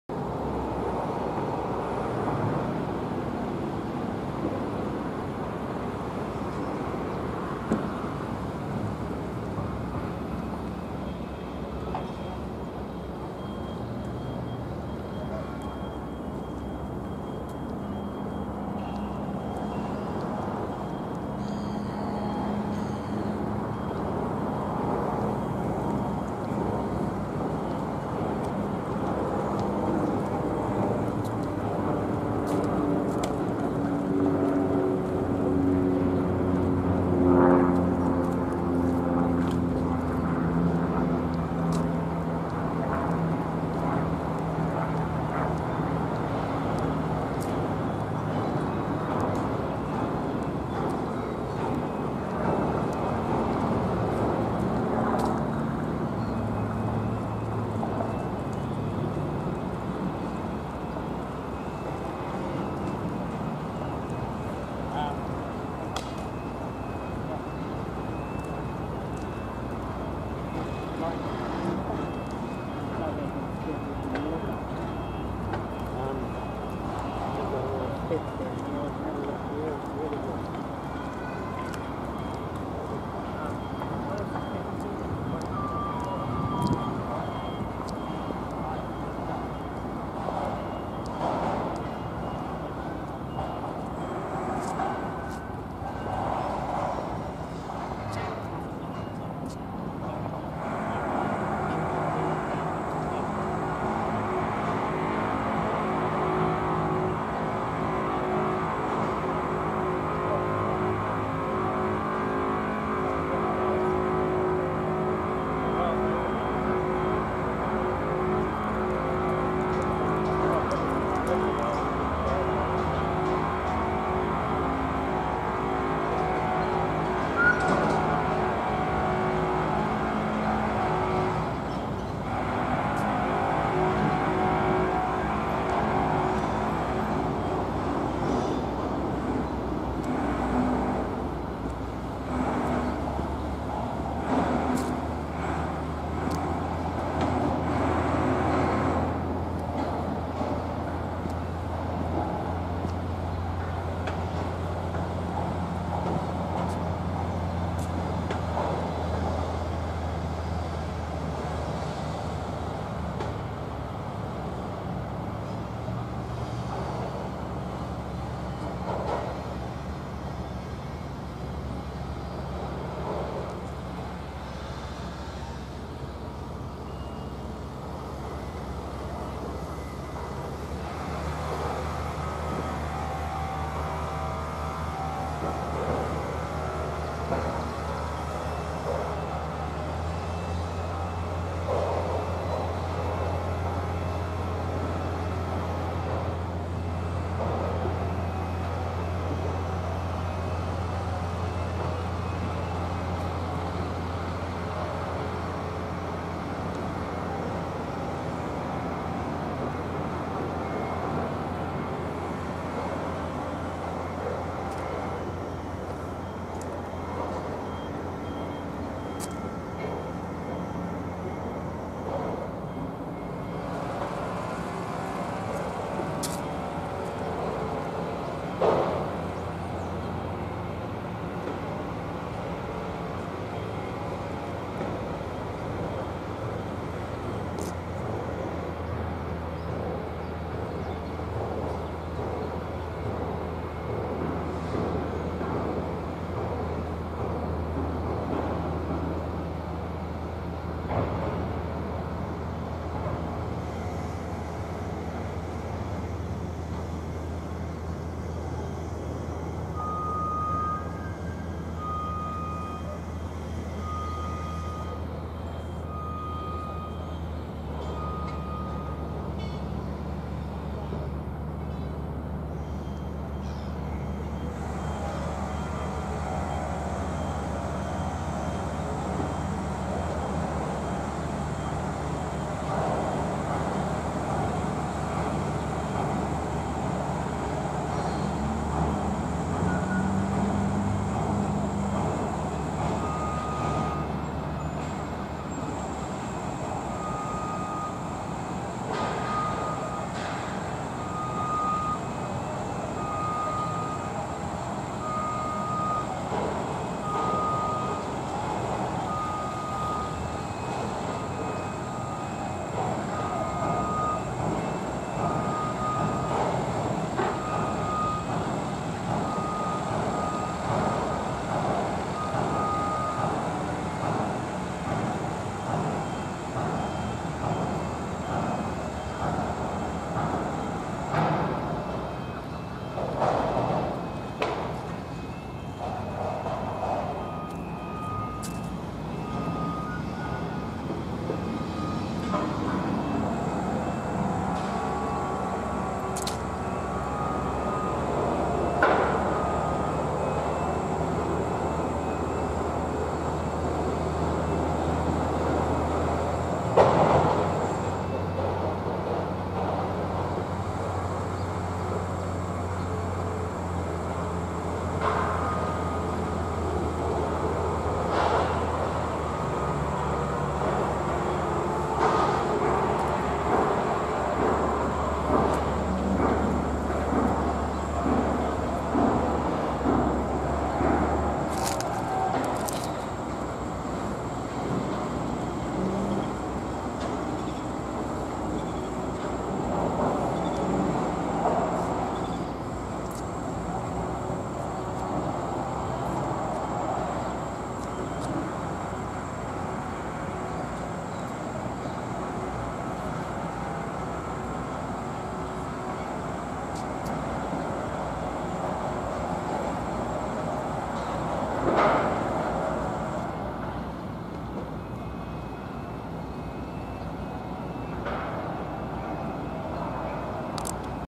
Oxford Tce. between Cashel Mall and Hereford St. (2016-05-18-15:38)
Walking across the Hereford St. Bridge from Remembrance Park to Oxford Tce. and along The Terrace to explore the new riverbank development on the Avon River. Recorded 2016, 18th of May at 3:38pm.